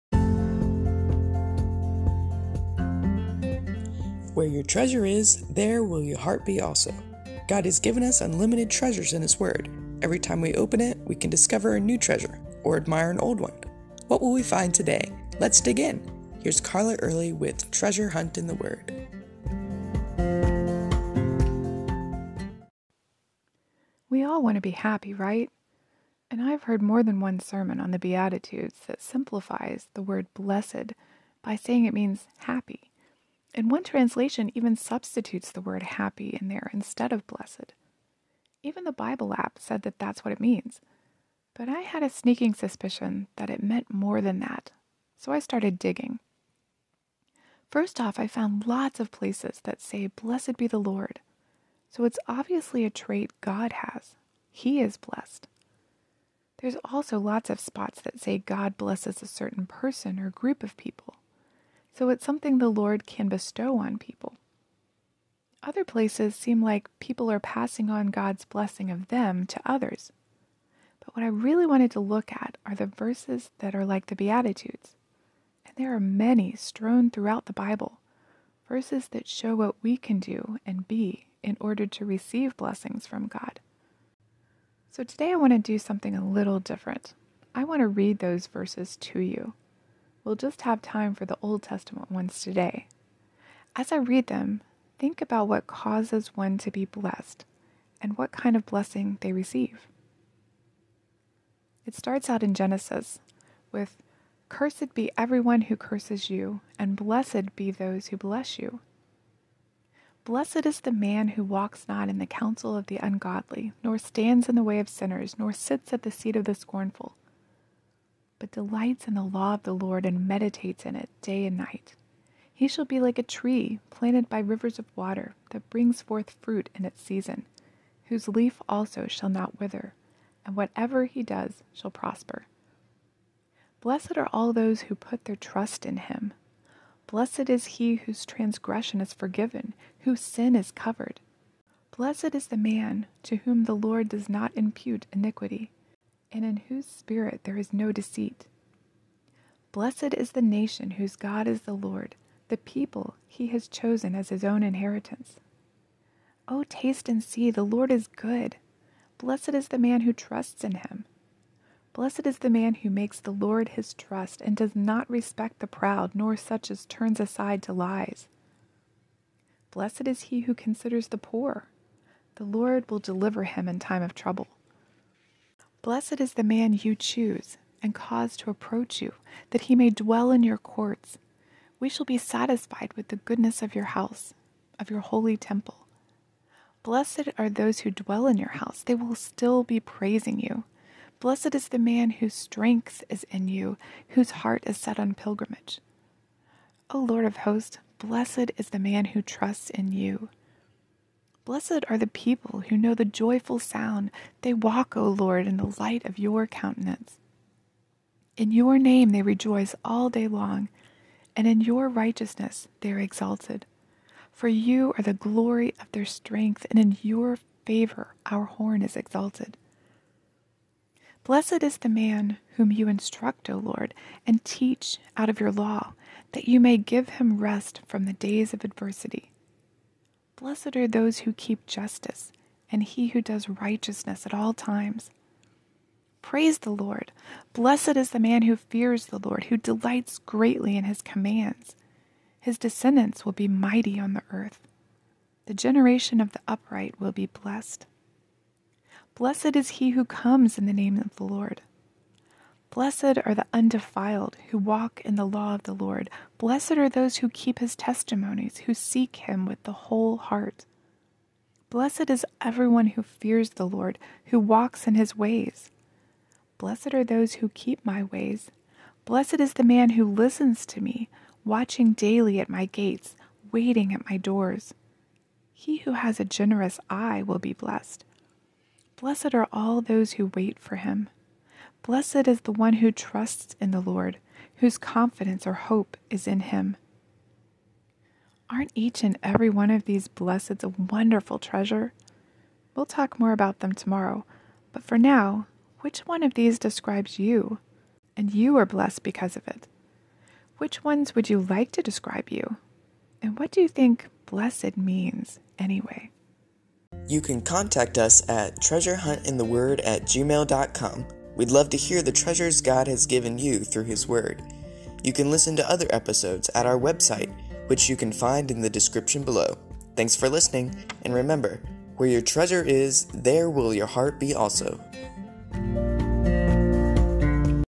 S3: E68: Blessed pt 1
Featuring Old Testament scriptures that are like the Beatitudes.